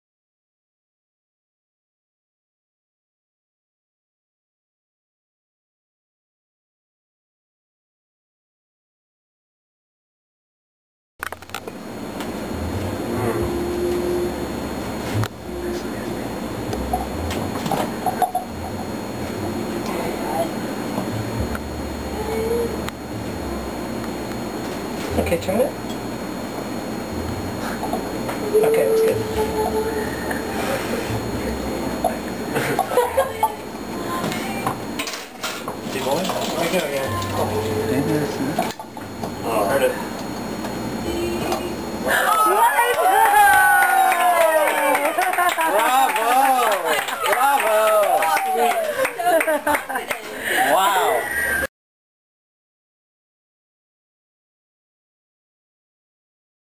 Improvised Port Tongs
Opening a bottle of 1966 Graham's Vintage Port wine with Port tongs made from a piece of heavy gauge wire.